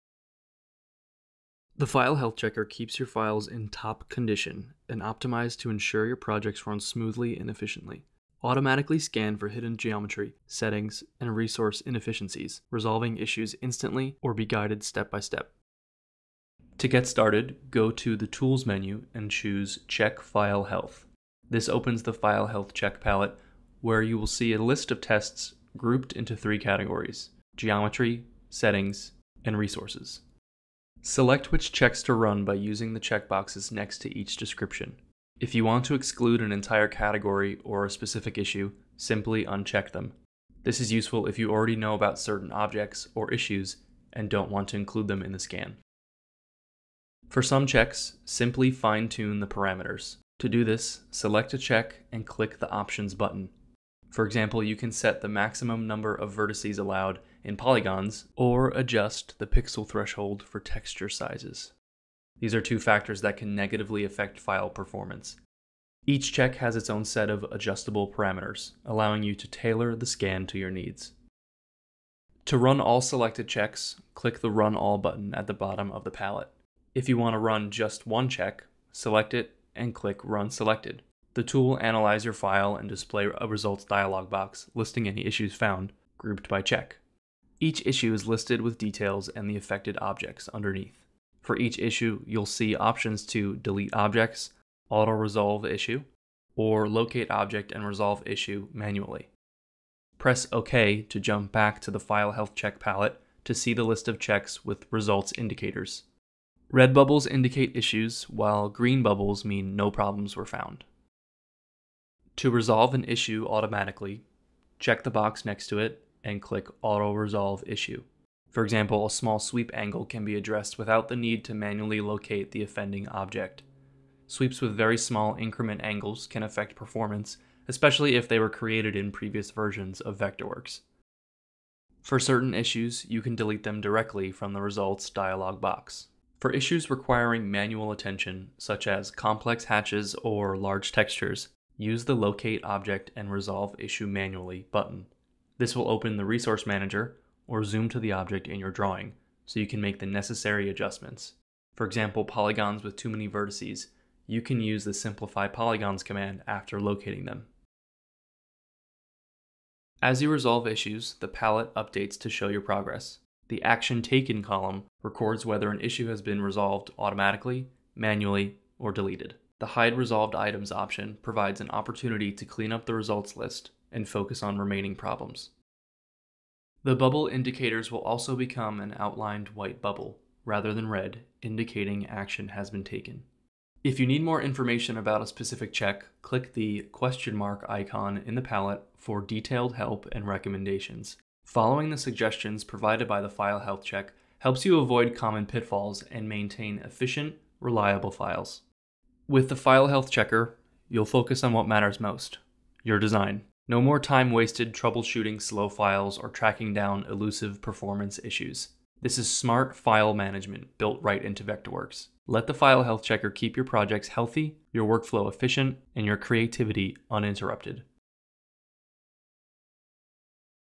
2026 File Health Checker
Keep your Vectorworks files in top condition with the File Health Check palette—available only to subscription users. Automatically scan documents for hidden geometry and resource issues, receive smart suggestions, and resolve inefficiencies in just one click. Spend less time troubleshooting and more time designing, knowing your projects are always optimized for peak performance. Discover how to get started with this video.